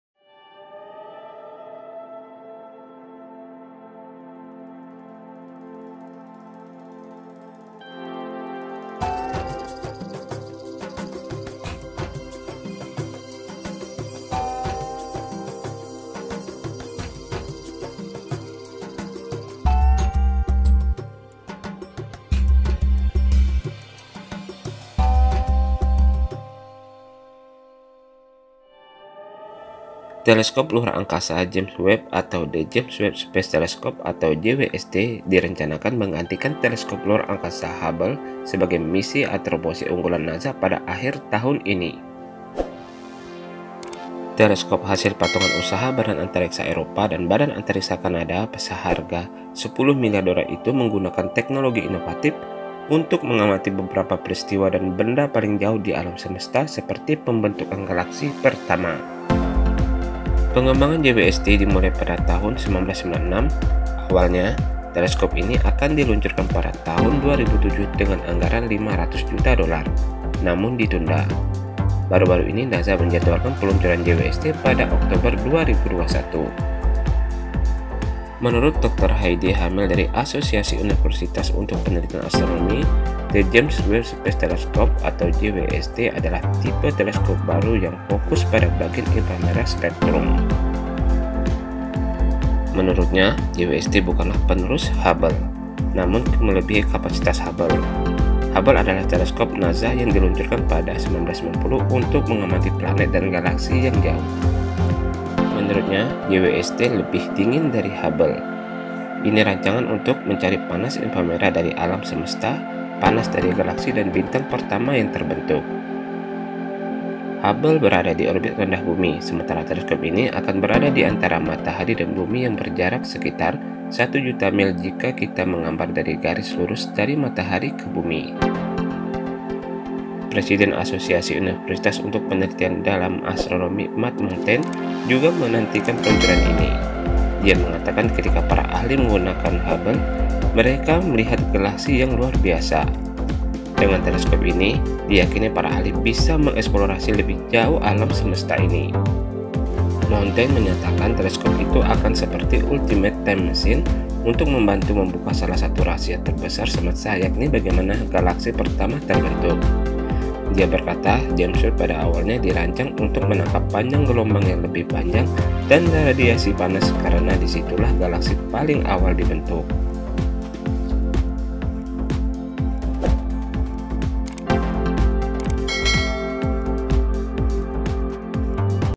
WAW !! TELESKOP SEHARGA 10 MILYAR USD, TELESKOP JAMES WEBB
Teleskop luar angkasa James Webb atau The James Webb Space Telescope (JWST) direncanakan menggantikan Teleskop Luar Angkasa Hubble sebagai misi Astropsi Unggulan NASA, pada akhir tahun ini. Teleskop hasil patungan usaha Badan Antariksa Eropa (ESA) dan Badan Antariksa Kanada (CSA), seharga US$10 miliar dolar itu menggunakan teknologi inovatif untuk mengamati beberapa peristiwa dan benda yang paling jauh di alam semesta, seperti pembentukan galaksi pertama. Pengembangan JWST dimulai pada 1996. Awalnya teleskop ini akan diluncurkan pada 2007 dengan anggaran US$500 juta, namun ditunda. Baru-baru ini NASA menjadwalkan peluncuran JWST pada Oktober 2021.